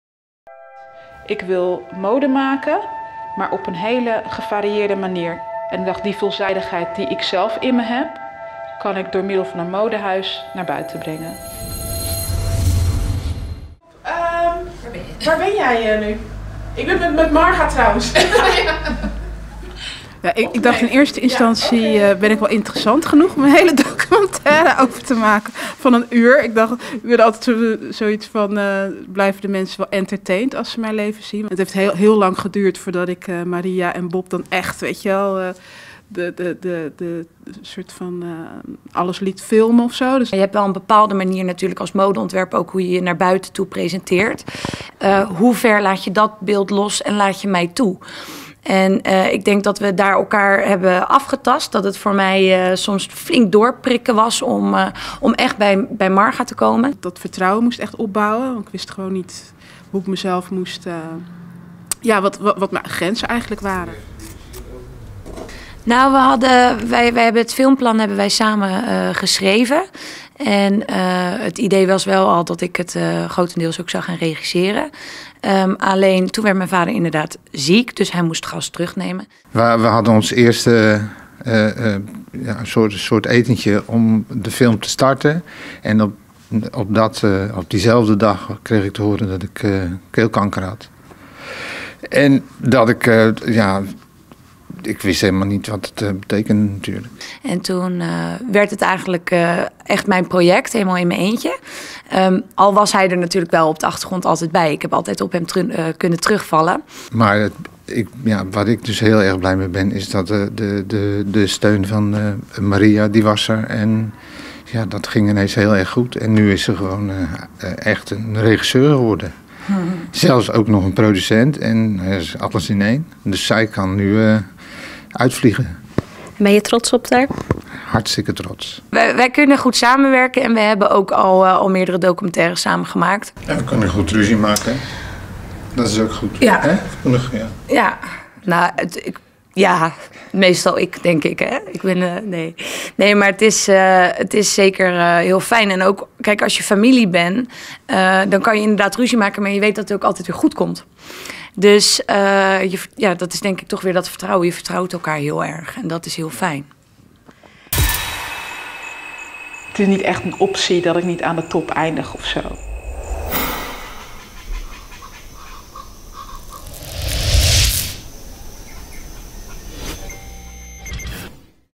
010nu - In gesprek met Maria Visser en Marga Weimans over making of Fashion House Marga Weimans
Fashion House Marga Weimans is het filmdebuut van Rotterdamse filmmaakster Maria Visser. Haar documentaire gaat in première tijdens het Filmfestival Rotterdam, dat vandaag van start is gegaan. Maria, haar vader Bob en ontwerpster Marga vertellen meer over het filmproces.